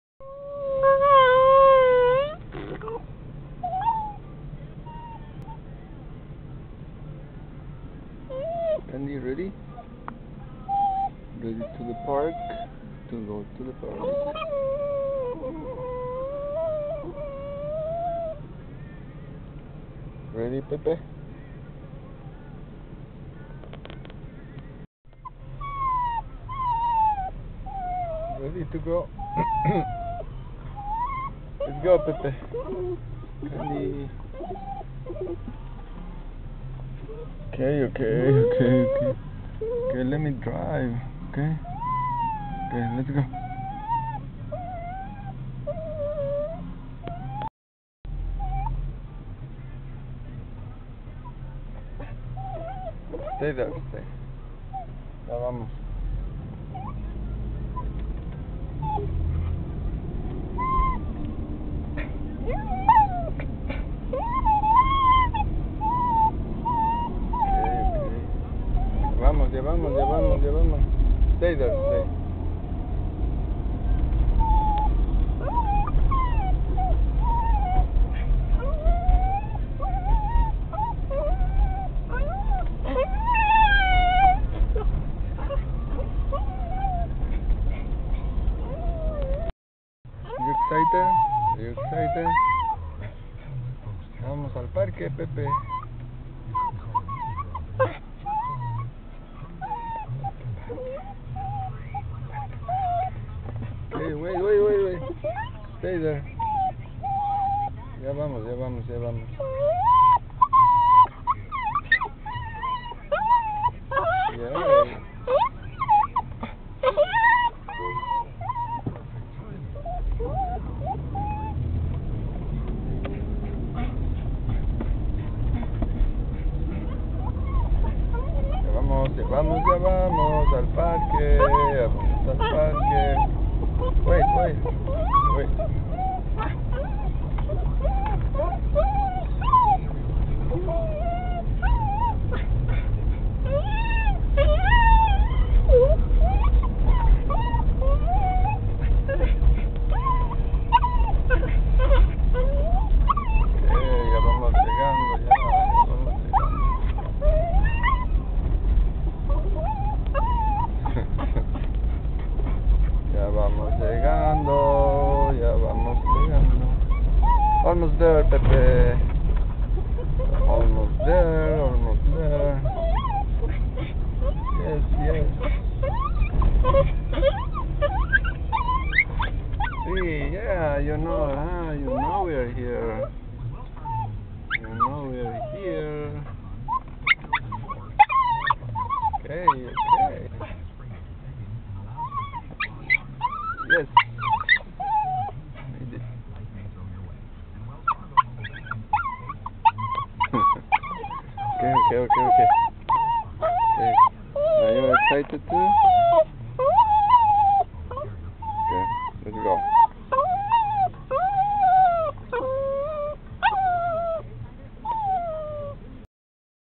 pepe is exited to go to the park
did you take your doggie to the park today...??